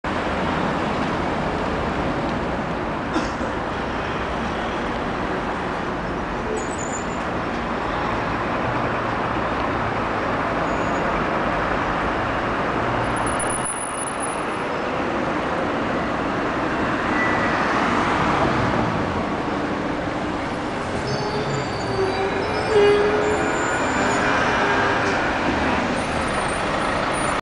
The Raizins at Piazzale Loreto in MILAN ITALY
The Raizins visit the place where they put mussolini 'up' for public exhibiton way back on April 29th 1945. The Raizins hope to learn from history. In war there are no real winners just victims.